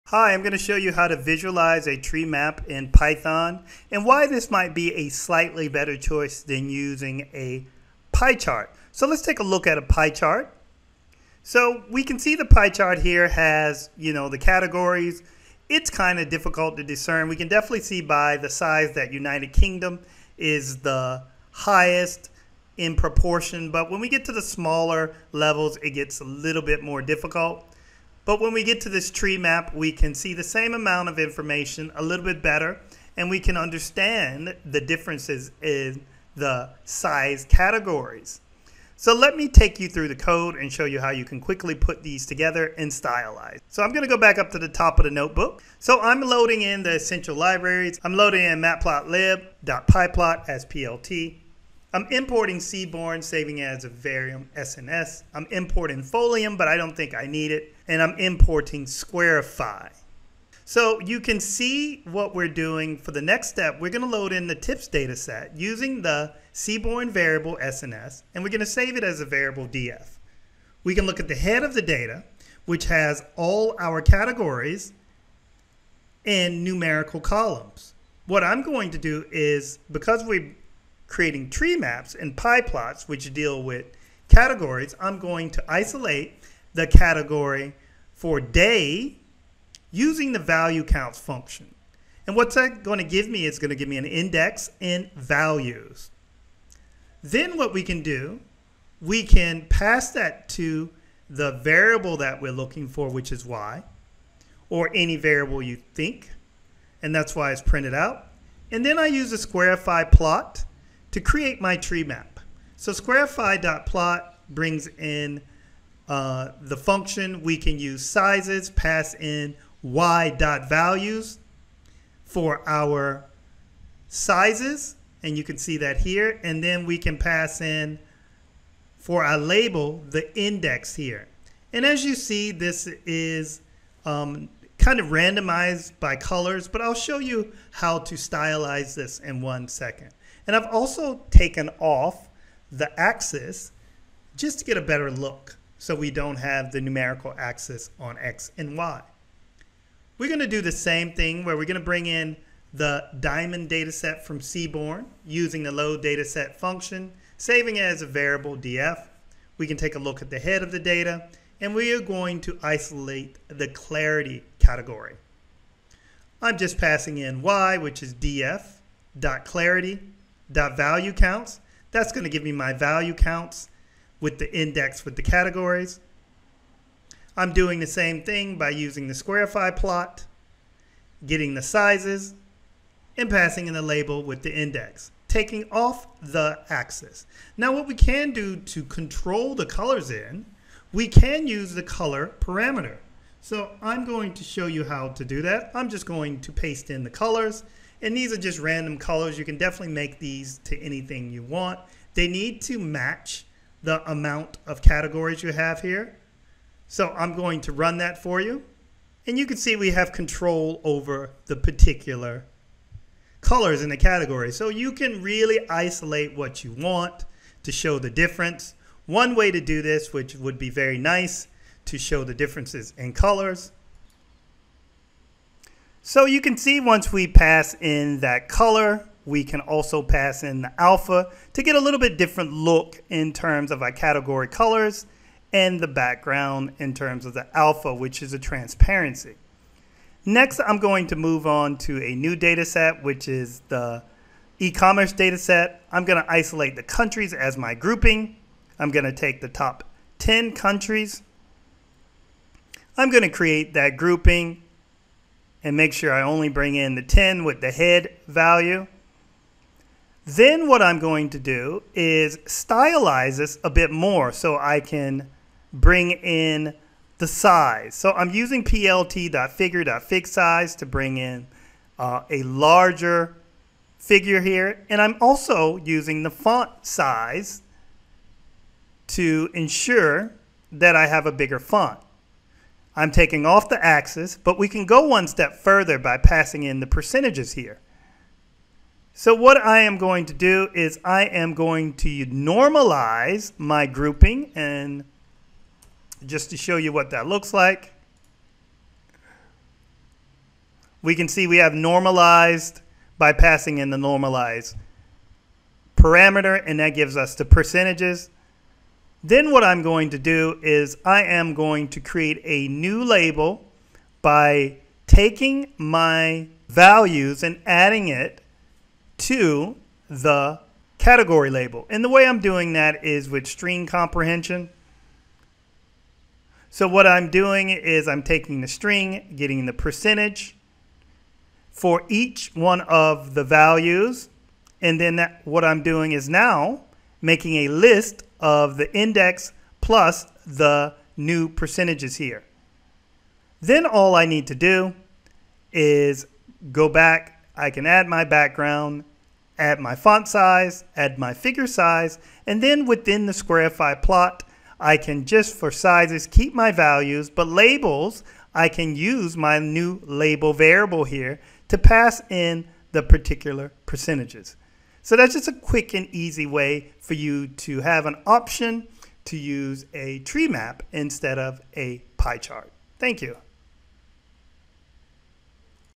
How to Make TREE MAPs in Python.
Learn how to quickly make tree maps using some simple code with Python. This will only require a few libraries to achieve. Learn how to create color maps and percentages for your categories.